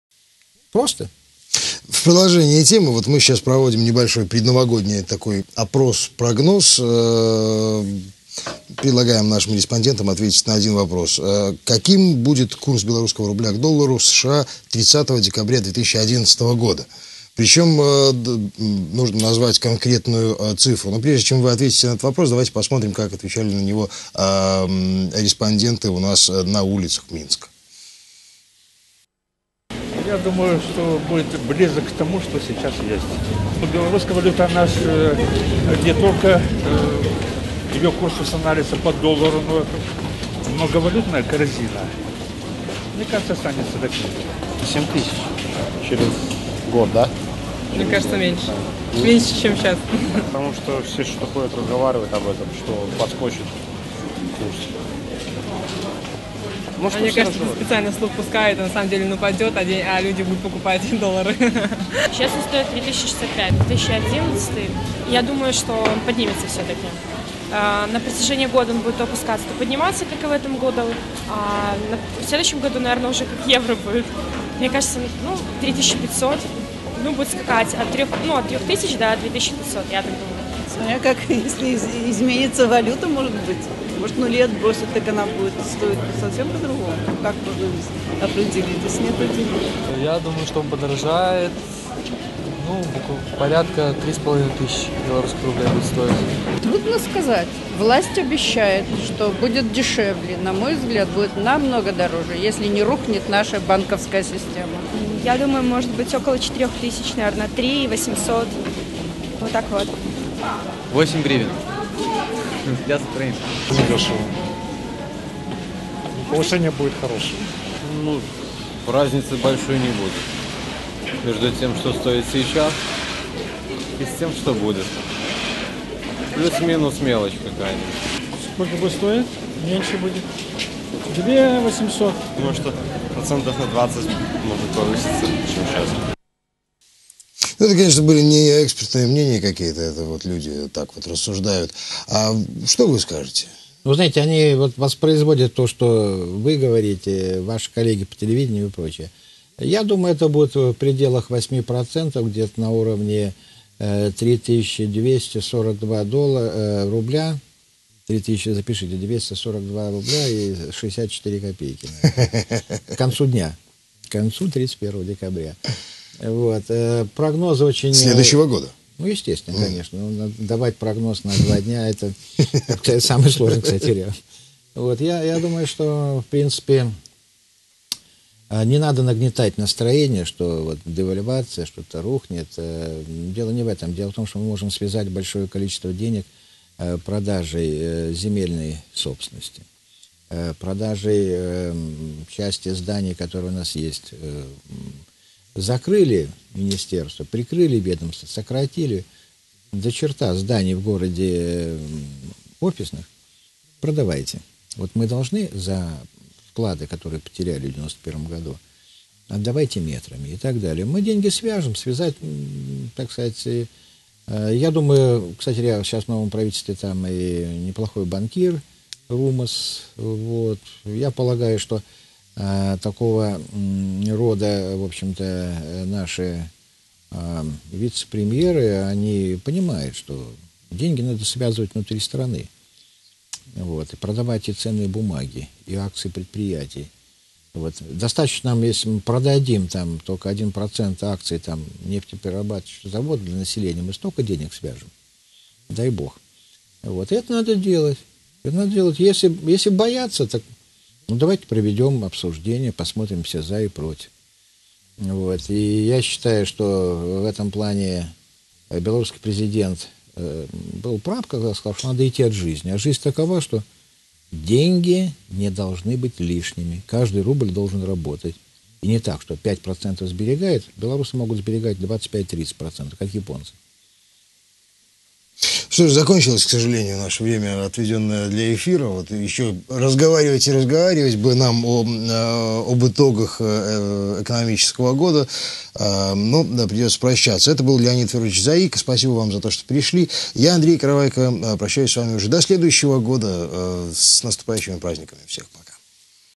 Экономические итоги года. (Часть 3)
Часть 3
В заключительной части программы гость студии Леонид Заико и рядовые минчане пытаются спрогнозировать валютный курс к концу 2011 года. Экономист призывает не нагнетать обстановку и поменьше говорить о девальвации.